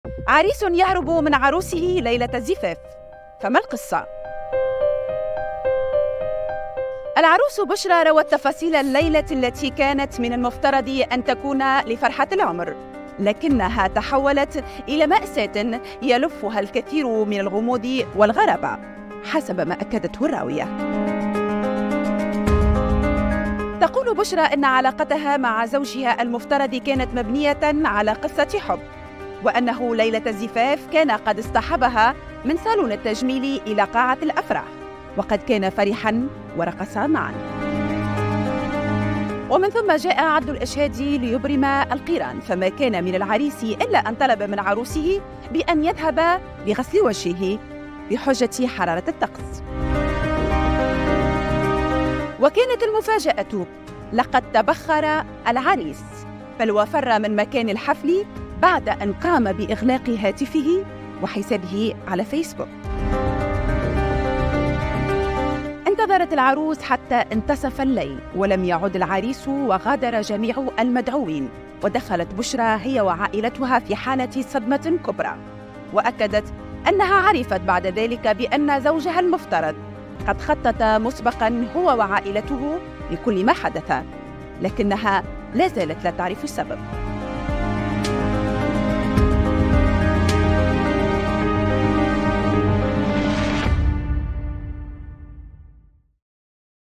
تركها في حالة صدمة.. عريس يفاجئ عروسه بهروبه في ليلة الزفاف
"فرحة العمر تحولت لماسأة".. عريس يفاجئ عروسه بهروبه في ليلة الزفاف
لمعرفة أحدث المستجدات.. عبر البث المباشر من هنا ⤵
وبإمكانكم متابعة العربية على منصات التواصل